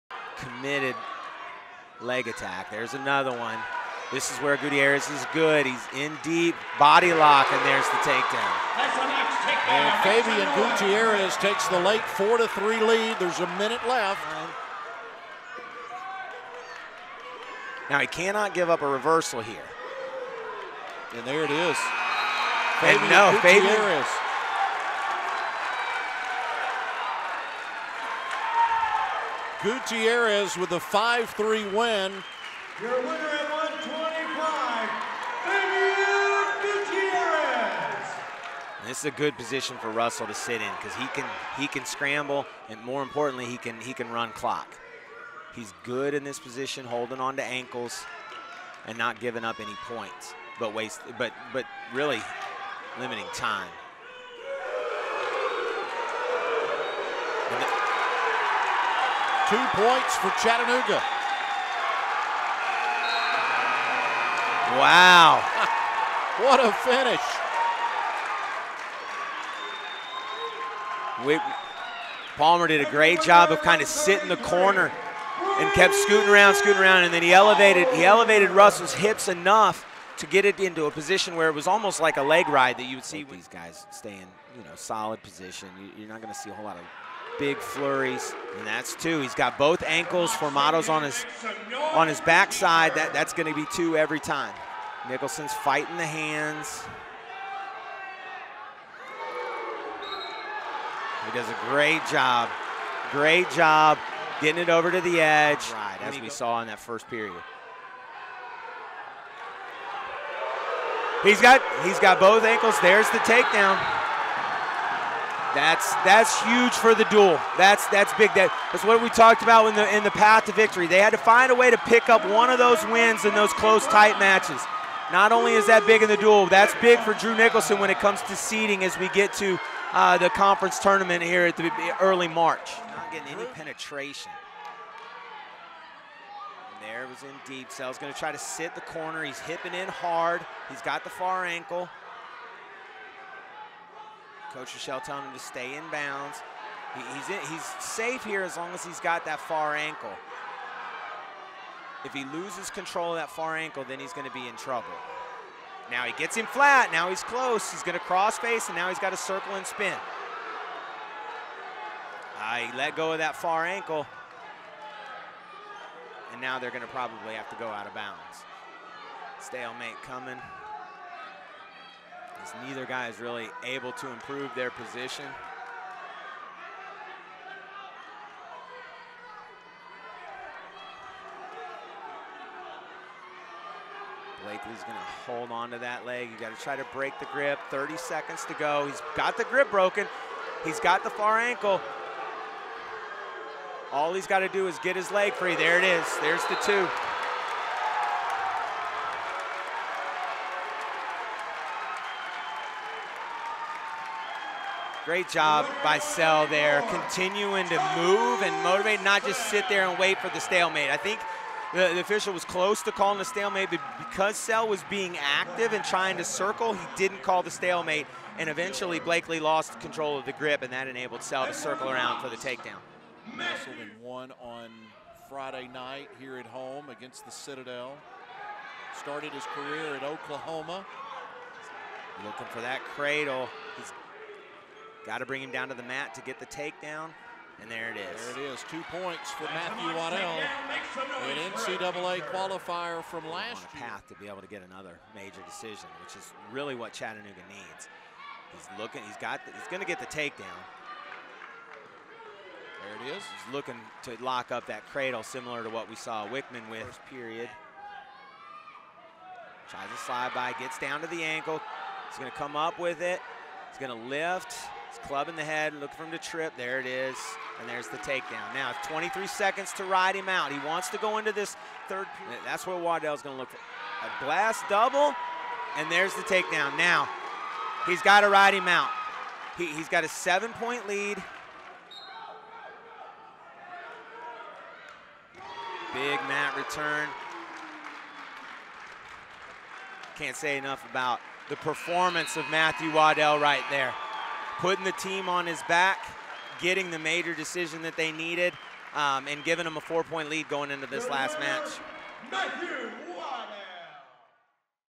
Highlights: Wrestling vs Appalachian State (2/6/2022)
Chattanooga Wrestling faced off against SoCon rival Appalachian State in Maclellan Gym. Mocs saw wins from Fabian Gutierrez (125), Brayden Palmer (133), Drew Nicholson (165), Thomas Sell (184) and Matthew Waddell (197).

Final score: Appalachian State 18, Chattanooga 16